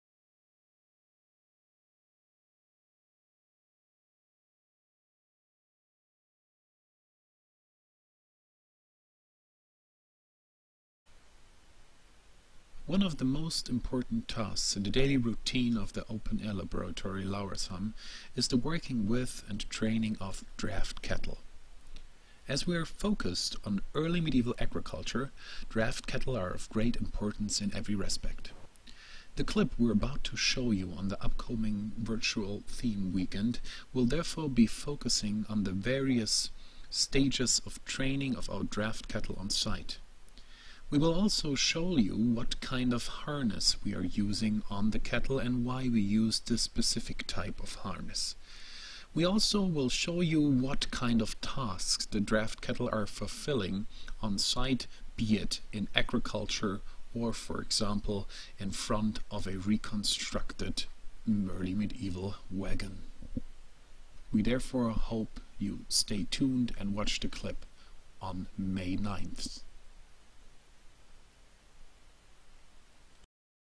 Teaser - Insights: Draft cattle Training at the Lauresham Open-Air Laboratory
The Clip "Draft cattle Training at the Lauresham Open-Air Laboratory" will be focussing on the training of and working with the draft cattle on site and will also explain what type of harness is used and why. This teaser will give you a short overview of what to expect from the Clip which will be published on May 9th.